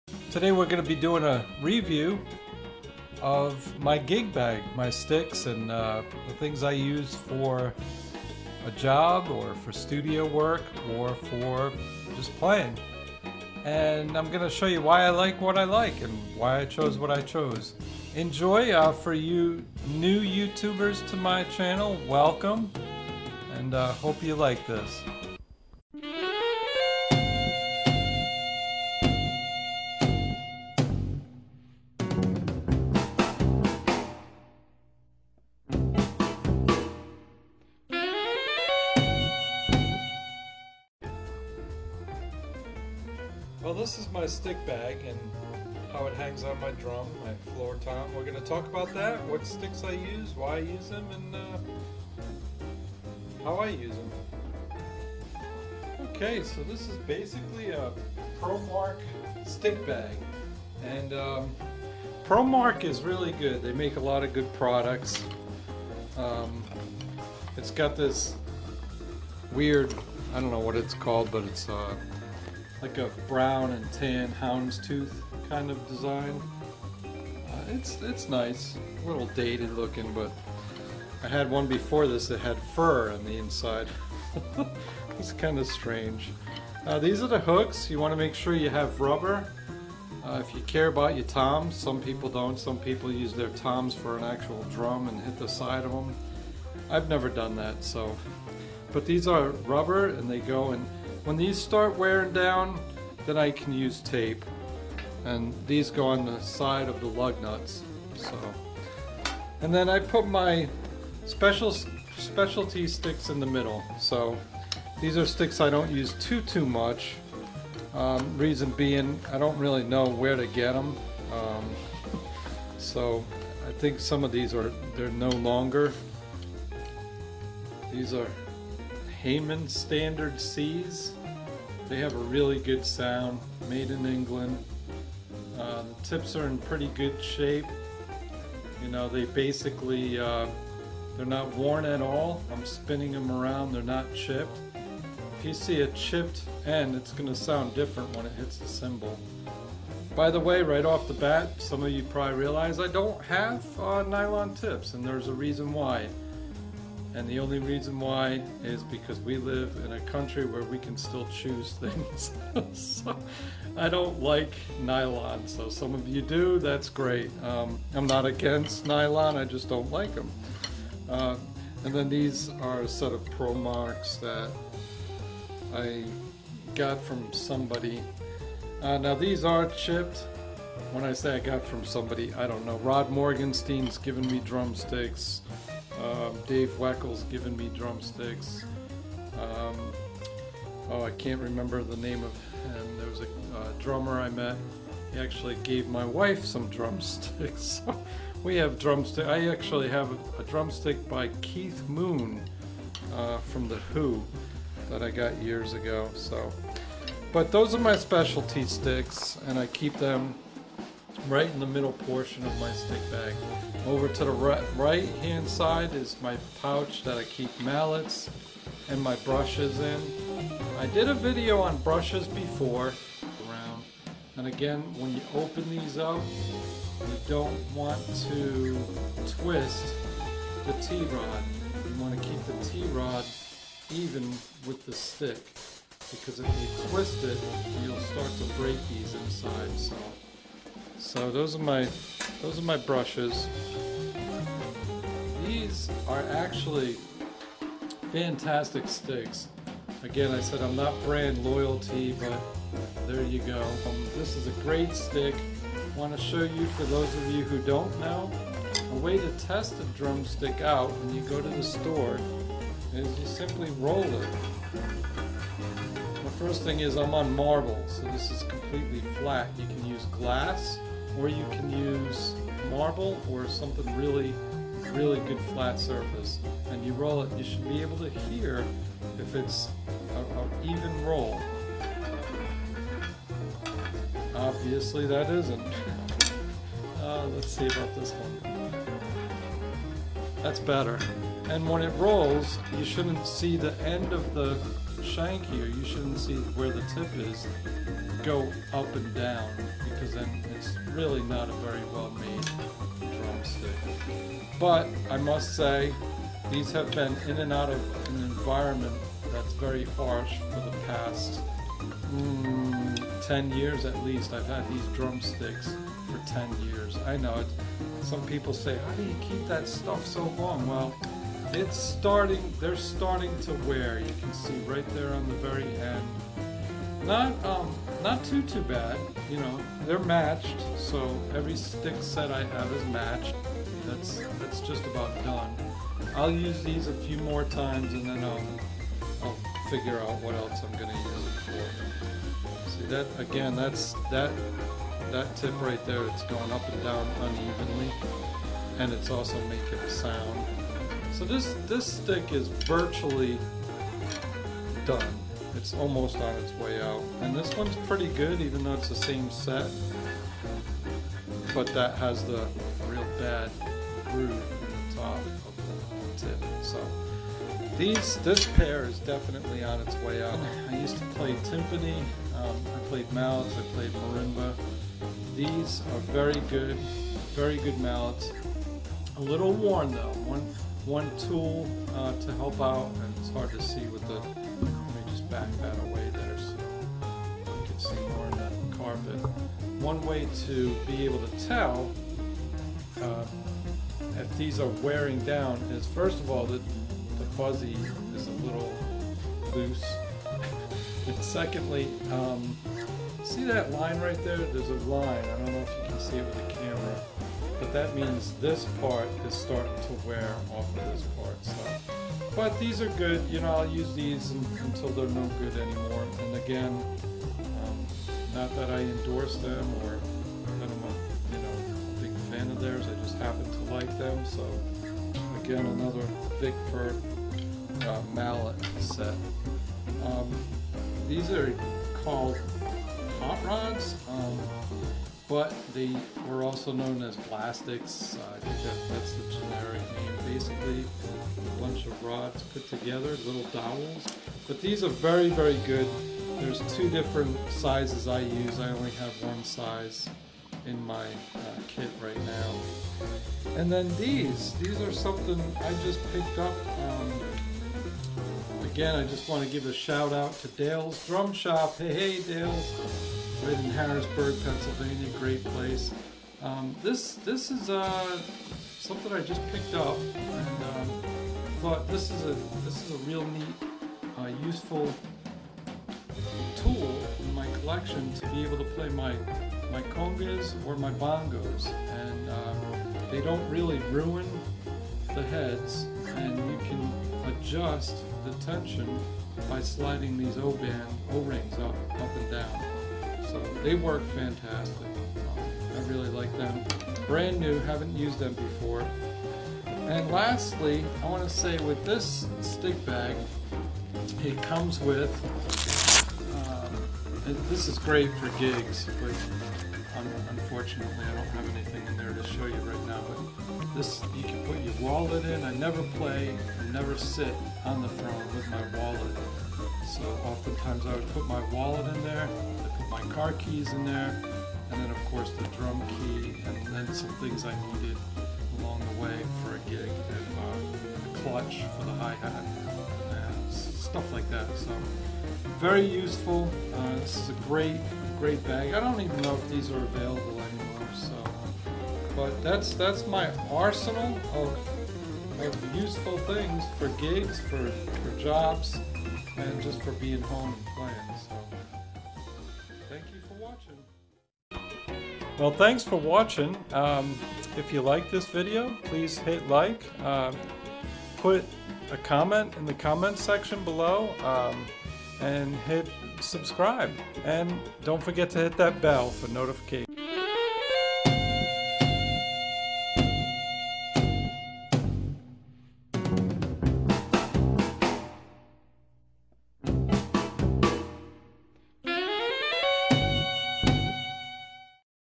Gig Gear Review | crossxpollination
This is all the gear that I use, and why I like it. That is all. *claps twice to dismiss viewers*

We don't talk about 1991.